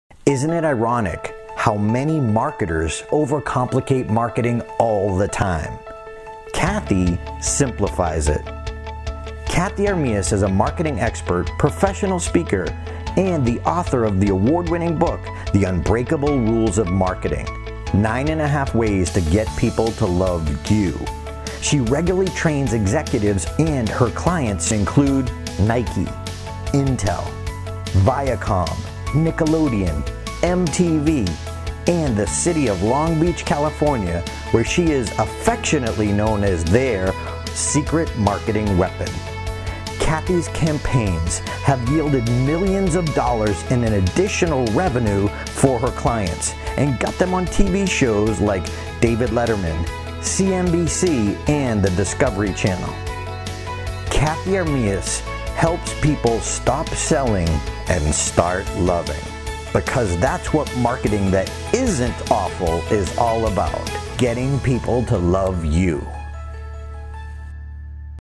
Cathey Armillas: marketer-speaker-author
Meet Cathey Armillas-marketing expert, international speaker, and co-author of the award-winning book, "The Unbreakable Rules of Marketing, 9 1/2 Ways to Get People to Love You."